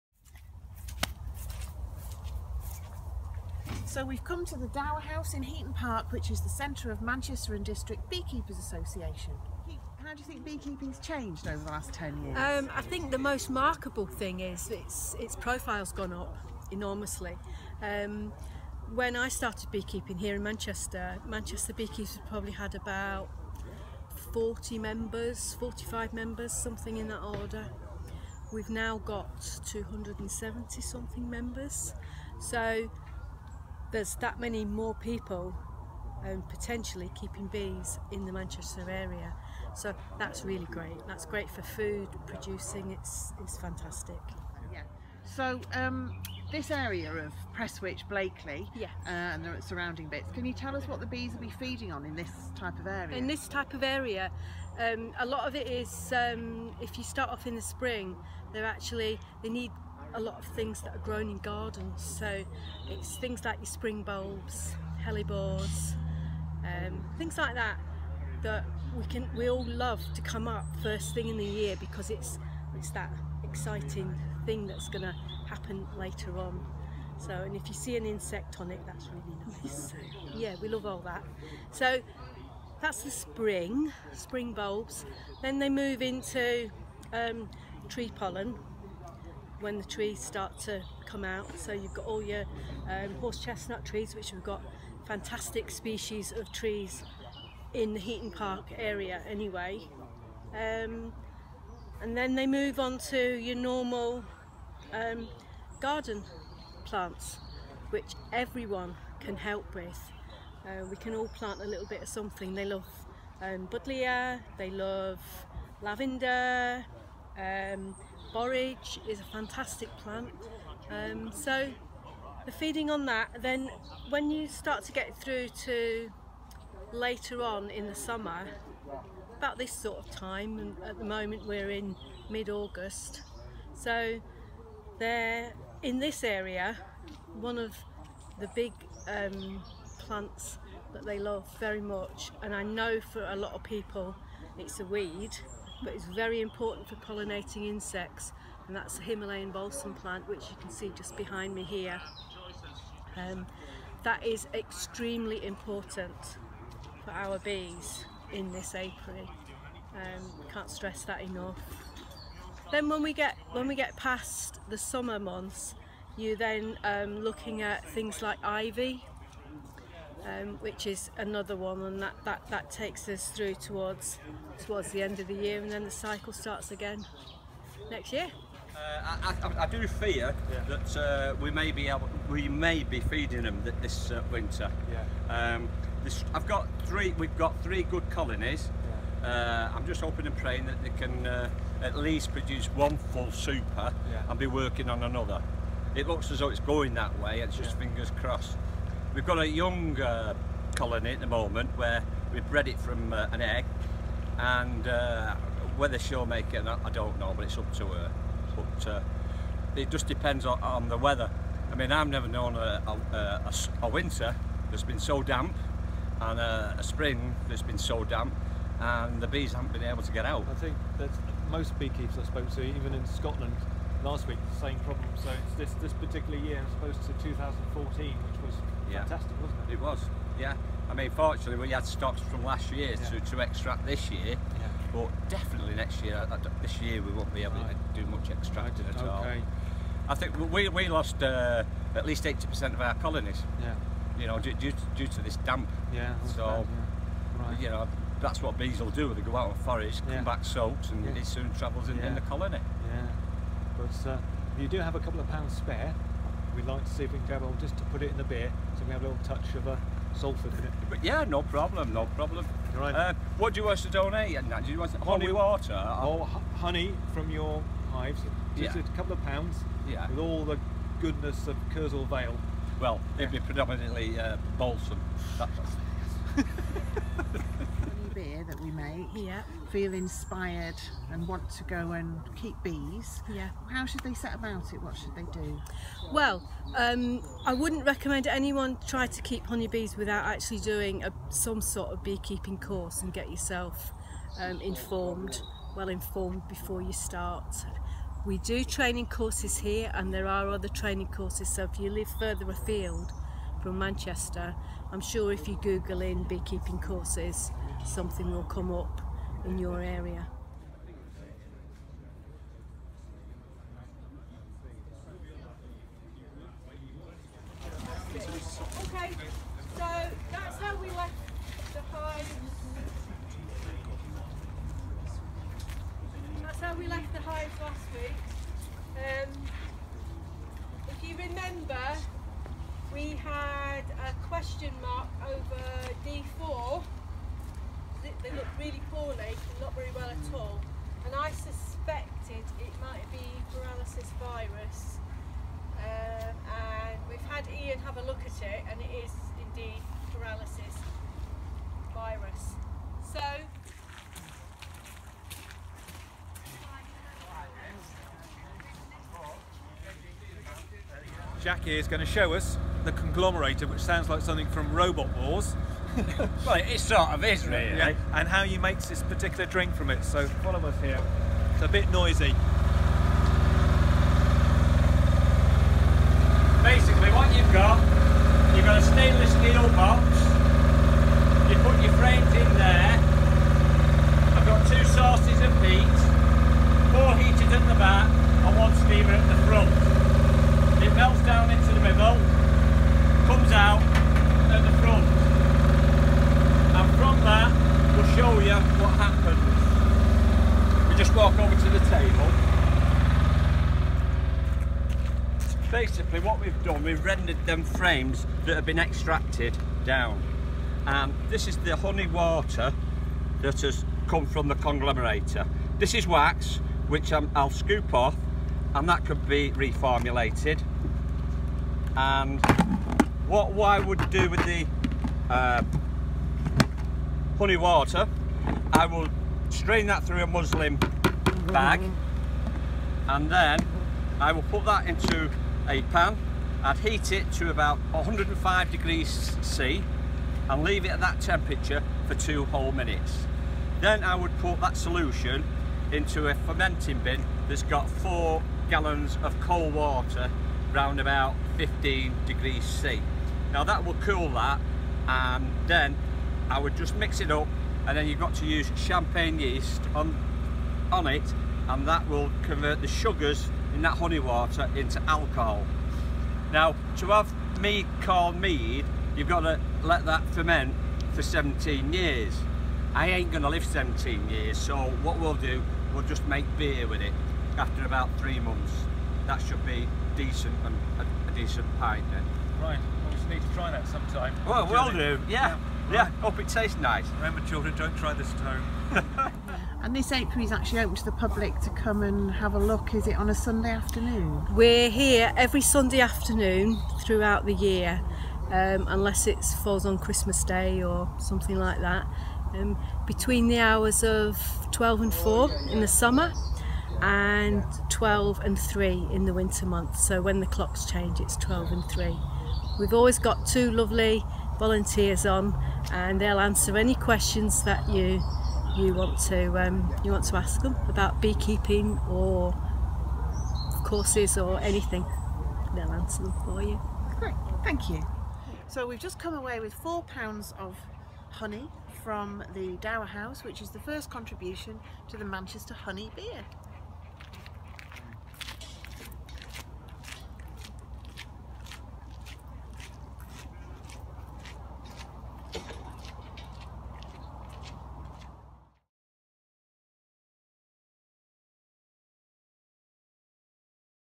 The Beekeepers of Heaton Park
Brew Wild meets the Beekeepers of Heaton Park. The Dower House is the HQ for Manchester & District Beekeepers Association. We pick up our first few pound of honey for the beer